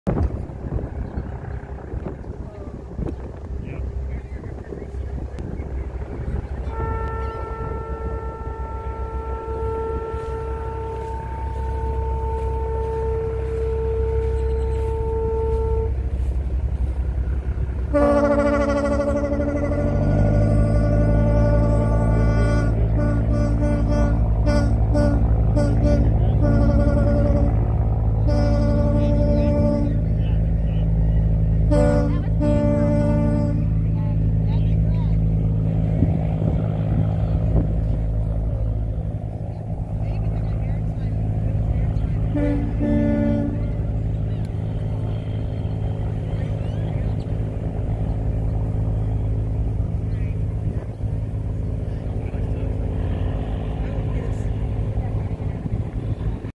Sound Off 2020
National Maritime Day tribute at pier 21 at the Port of Galveston on May 22, 2020.